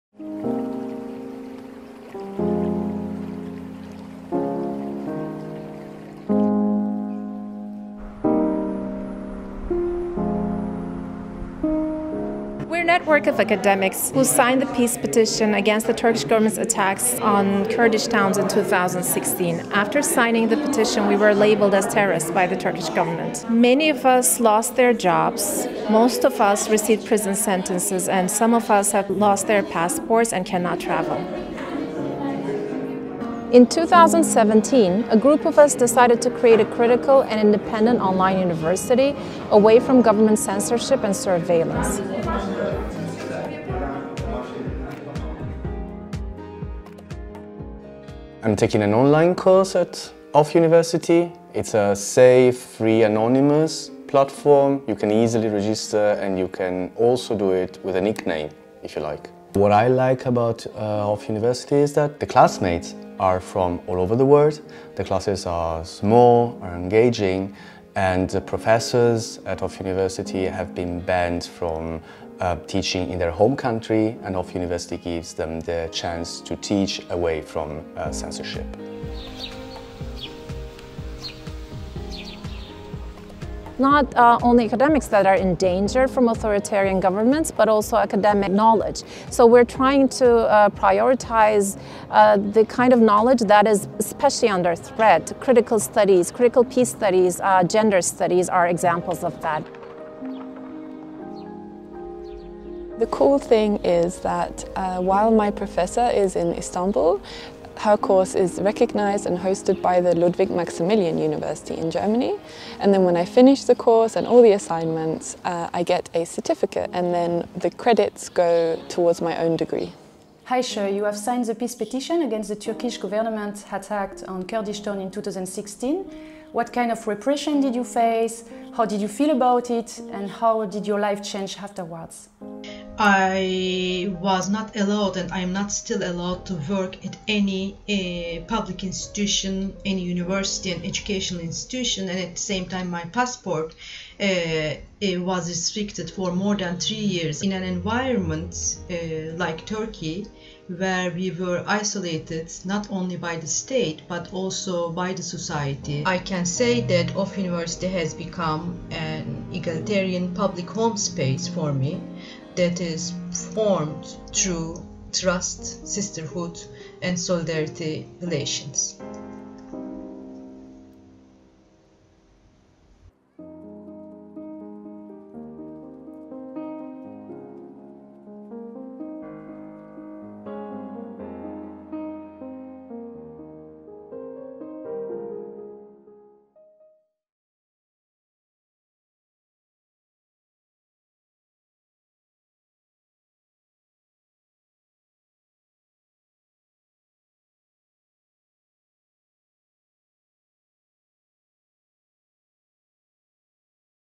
Online-Kurse mit Off-University
Off-University bietet kostenfreie Onlinekurse von politisch verfolgten Wissenschaftlerinnen und Wissenschaftlern an.